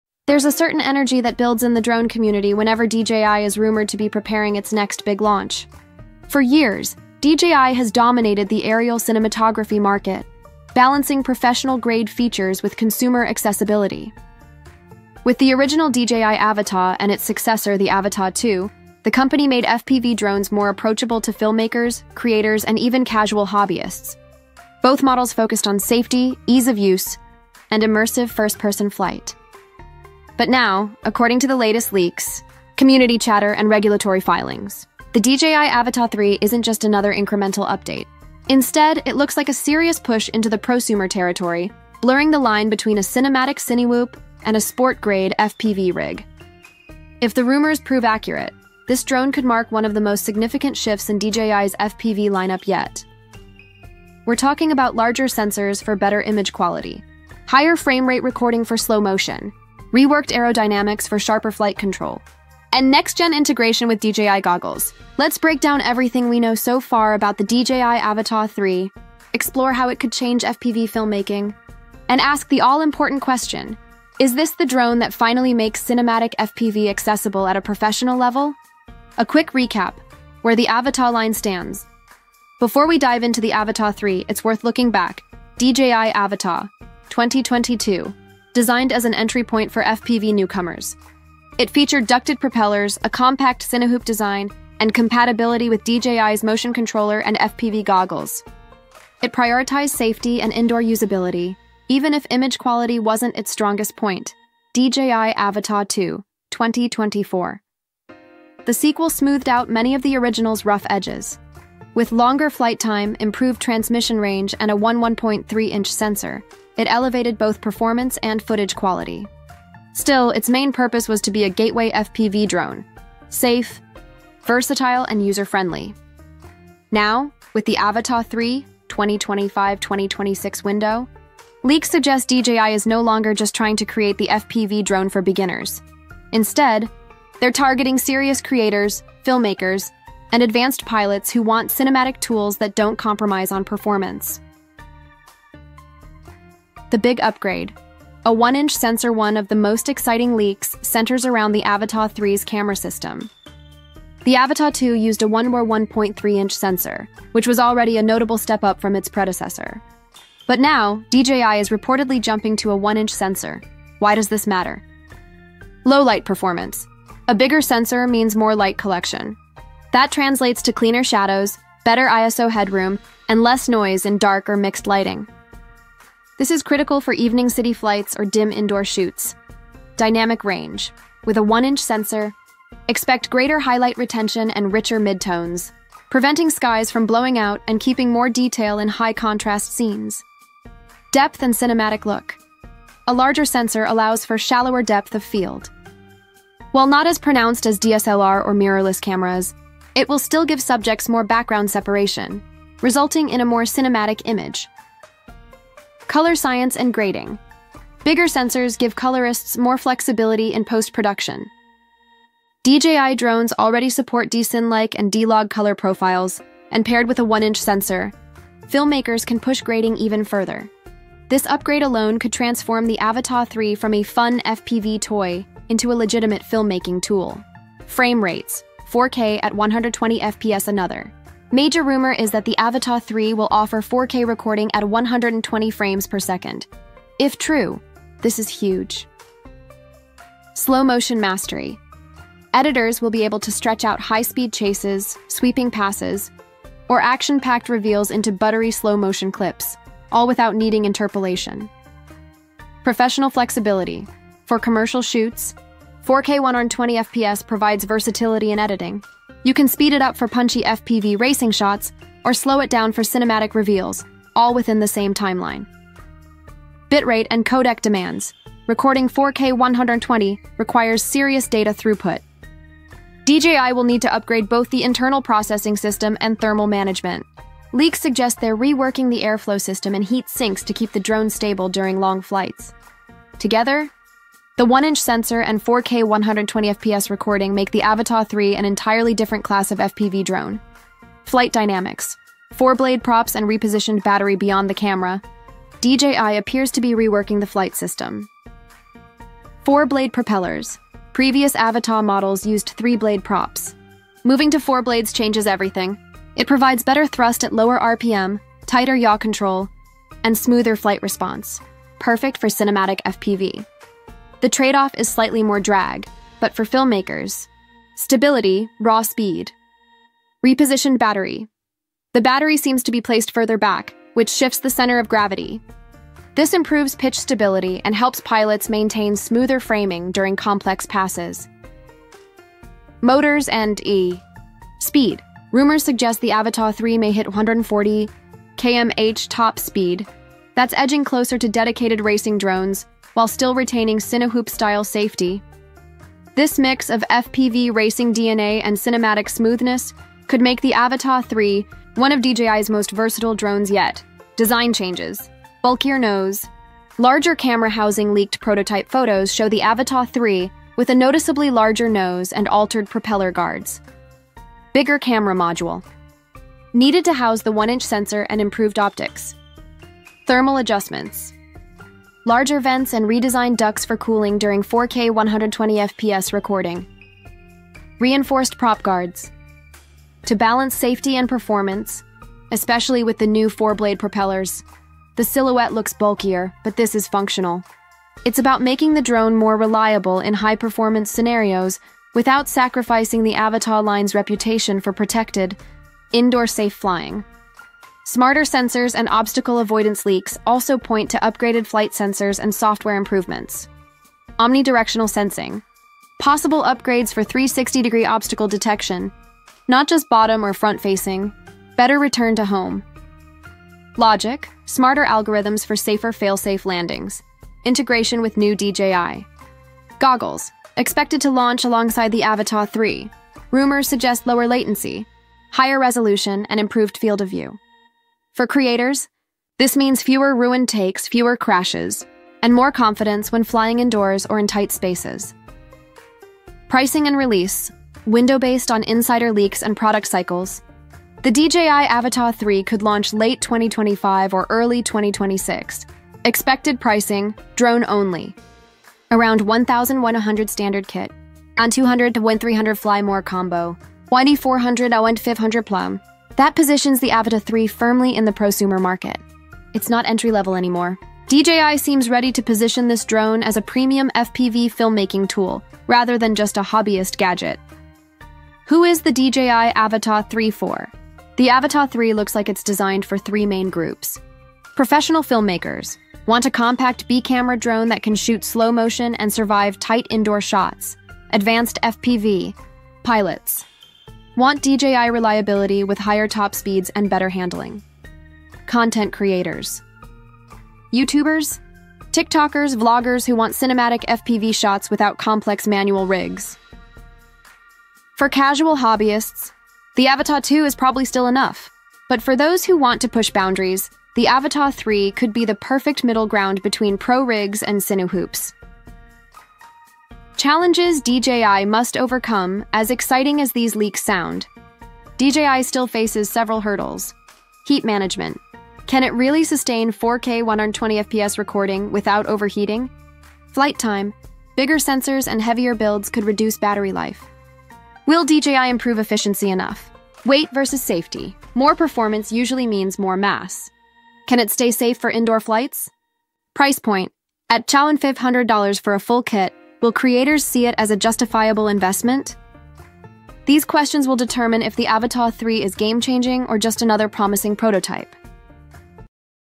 DJI Avata 3: The Fastest FPV Drone in DJI’s Lineup
The rumored DJI Avata 3 is expected to be a groundbreaking, high-speed First-Person View (FPV) "cinewhoop" drone, designed to deliver both adrenaline-fueled flight and professional-grade aerial cinematography. Poised to be the fastest FPV drone in DJI's consumer lineup with a rumored top speed of up to 140 km/h (87 mph), it is built around a compact, durable design with integrated propeller guards.